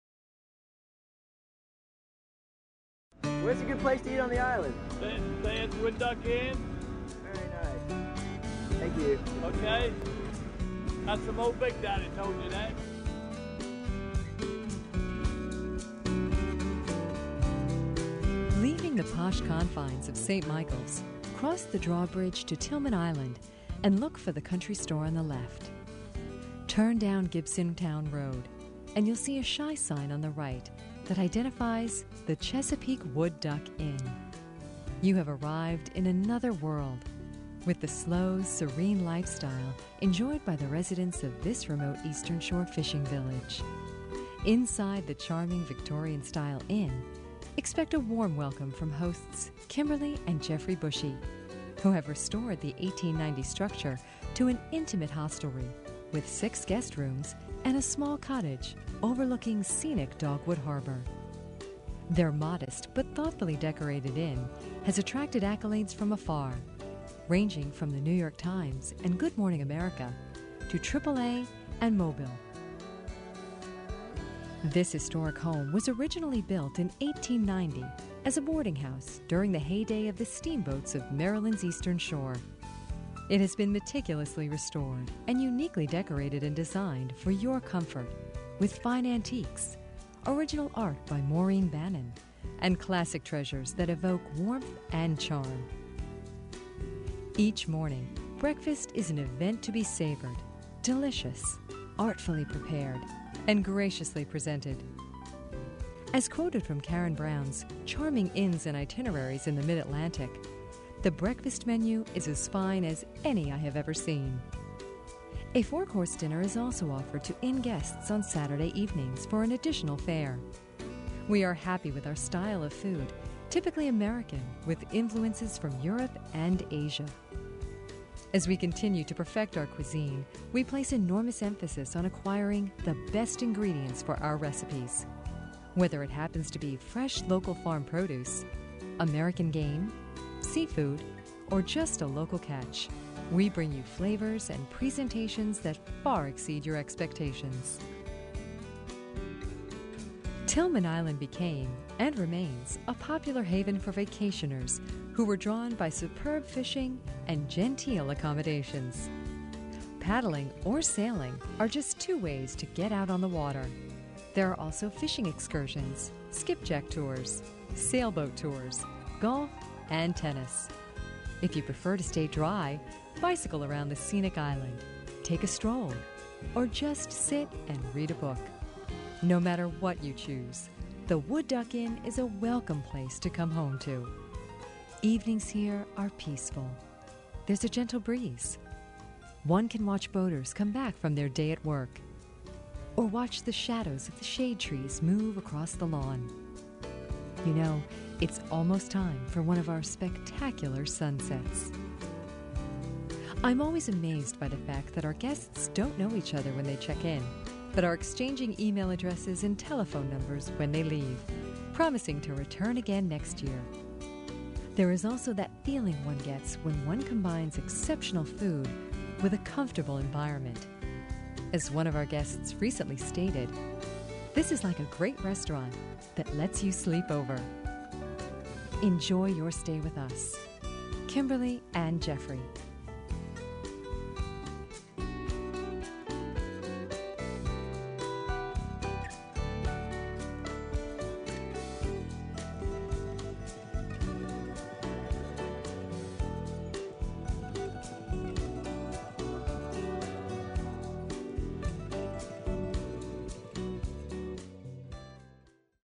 Chesapeake Wood Duck Inn (Tilghman, MD)
Put your feet up and enjoy the relaxed ambiance of the charming Chesapeake Wood Duck Inn lovingly restored with impeccable taste and care.Your hosts, Kimberly and Jeffrey strive to exceed your expectations so you'll have an unforgettable experience. Tilghman Island, only three square miles, is easily accessible to St. Michael's and Easton yet remote enough to stem the tide of everyday cares. When you cross over the drawbridge you'll be greeted by the 19th century charm of a true waterman's village.